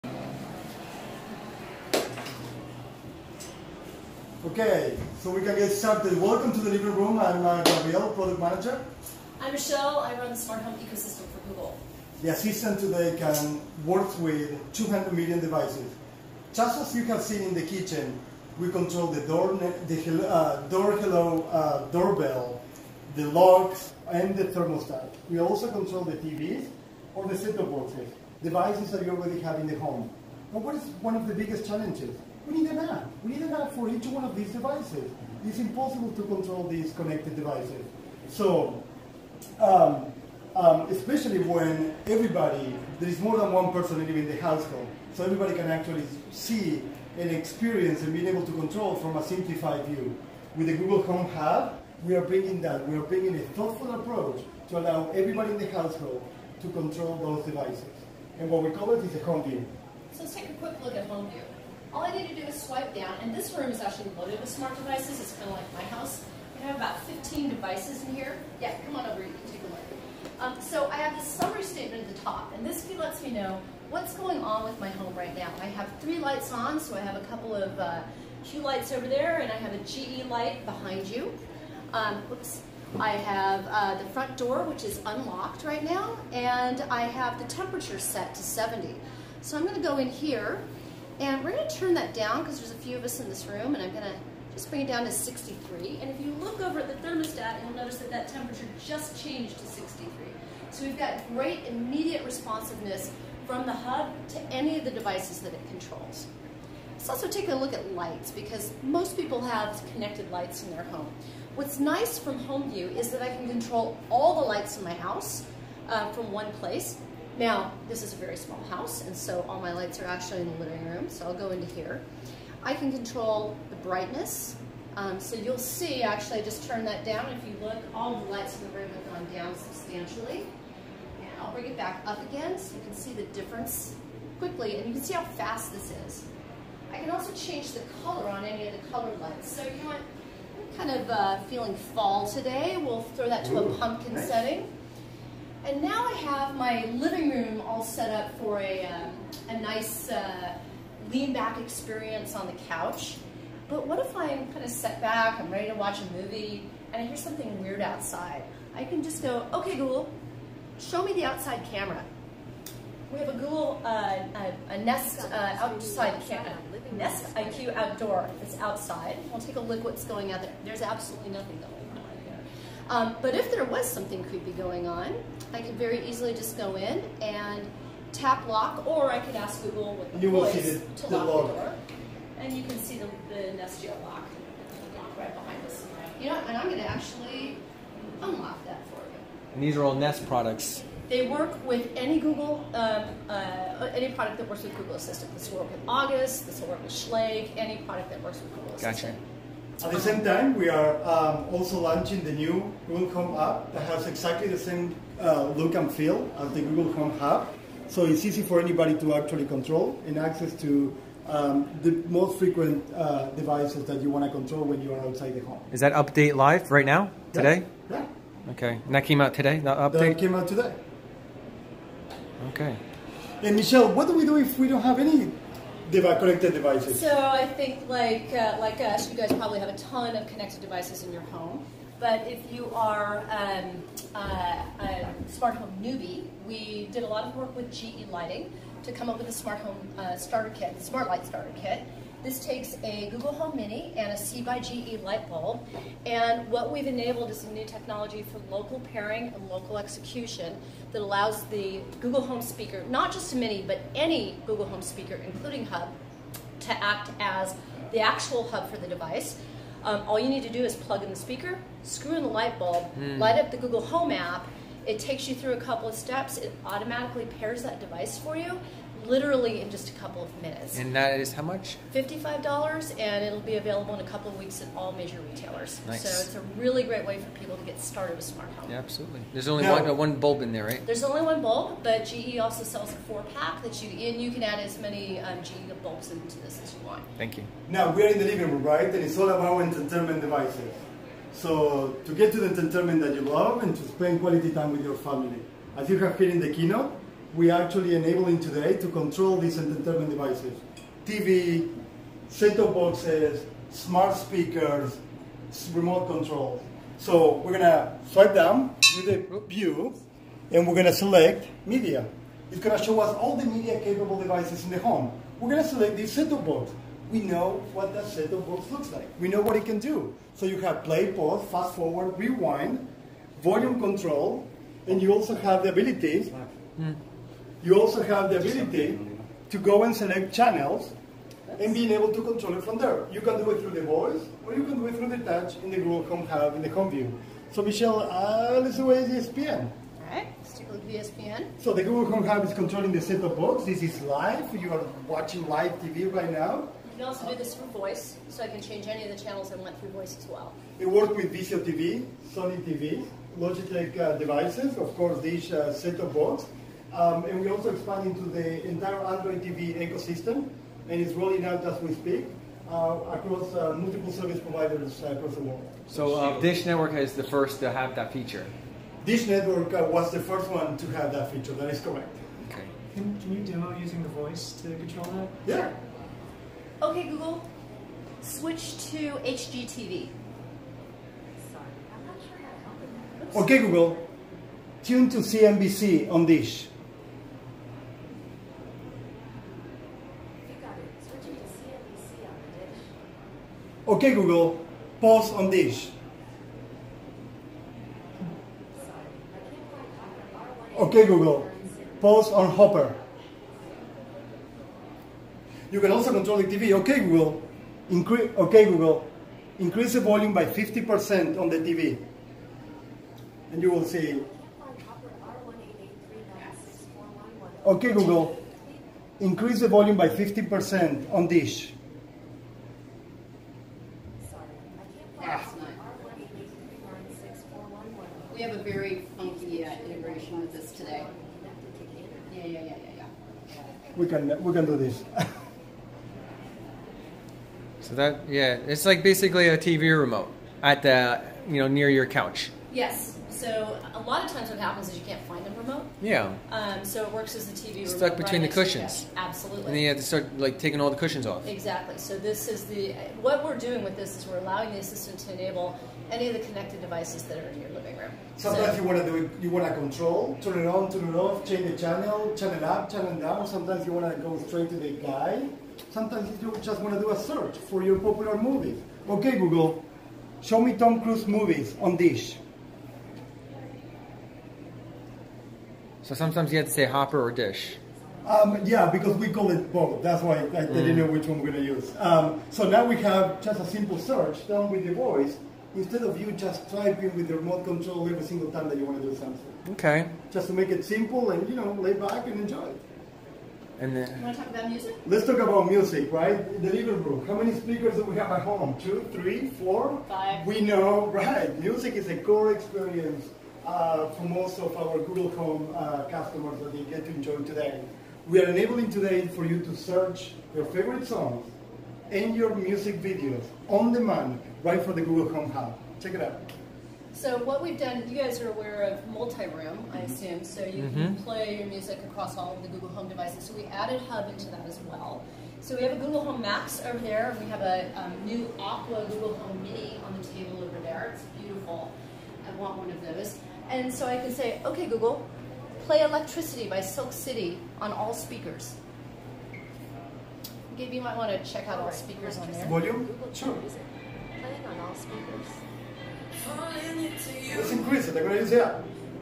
How to use The New Google Home Hub speaker, live Demo !
Learn how to use the Google Home Hub $150 works with Google home app on your android phone or iPhone and is the hub for your home automation. sponsored by google